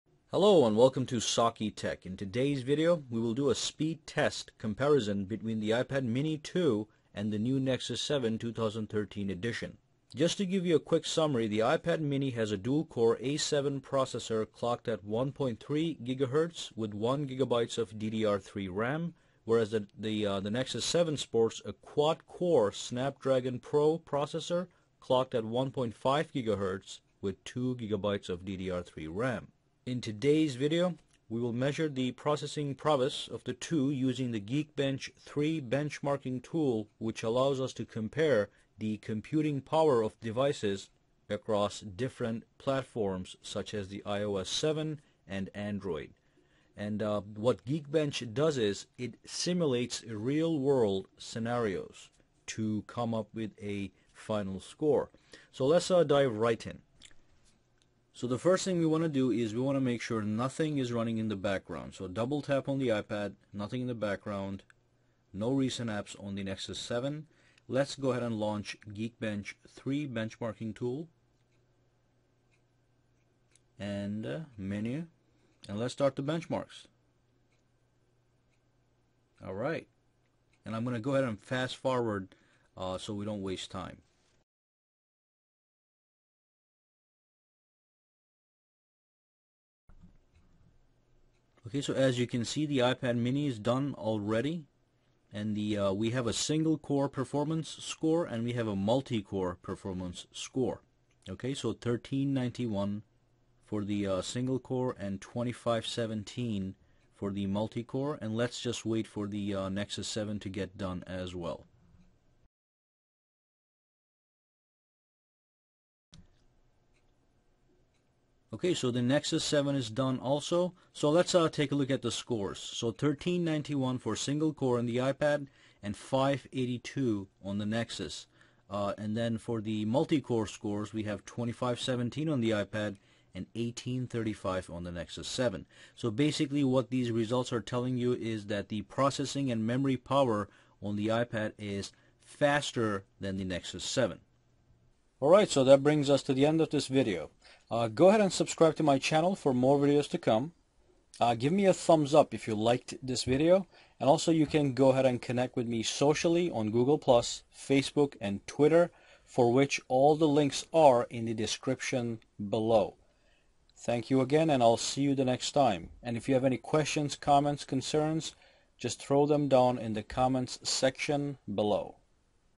iPad Mini 2 (Retina Display) vs New Nexus 7 2013 Speed Test Comparison
In this video, we will do a processing speed comparison between the iPad Mini 2 with Retina Display vs the New Nexus 7 2013 Edition by using the geekbench 3 benchmarking tool. Gekkbench simulates real-world scenarious to produce meaningful scores which are comparable on cross-platforms.